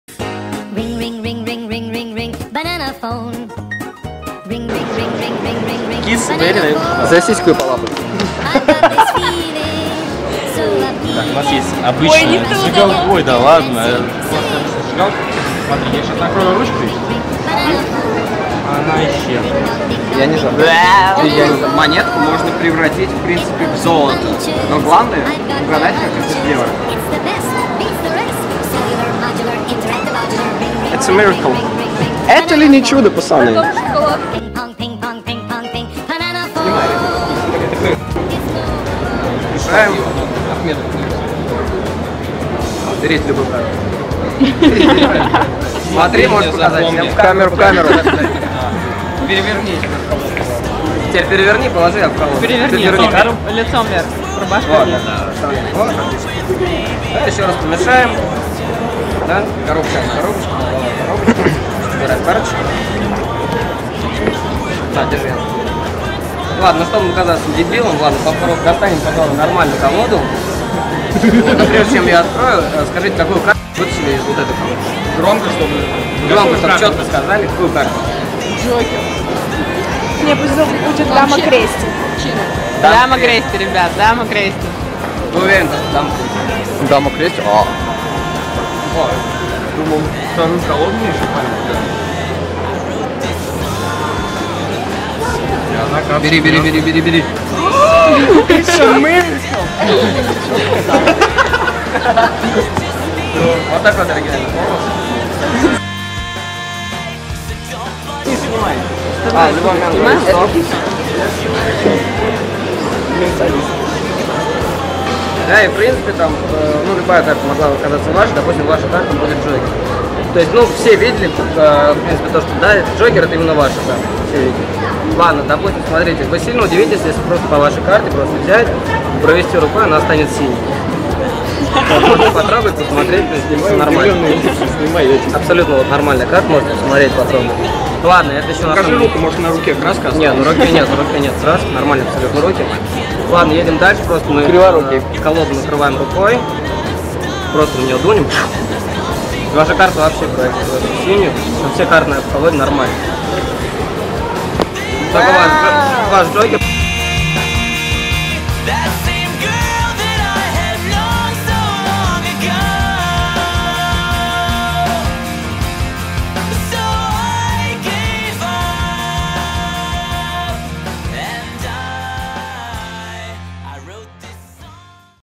Banana Show #4
- наша группа в контакте. Вступите, отвечайте на опросы и будьте в курсе событий.

Валентин-монтажер, автор канала на YouTube
Сергей-фокусник, автор идеи. Фокус с увеличением монеты в начале.
Никита-все основные фокусы на видео.
Дмитрий- фокус с исчезновением зажигалки в начале видео.

Канал посвящен фокусам и прочей магии и иллюзиям.

Выпуск четвертый.